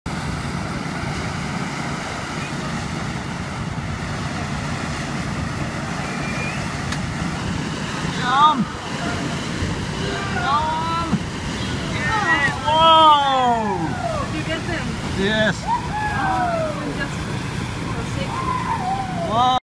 JumpingIntoWaikato20180609
Crazy people jumping off Hamilton bridge into Waikato on 9 June 2018. Seen from Waikato Explorer cruise boat.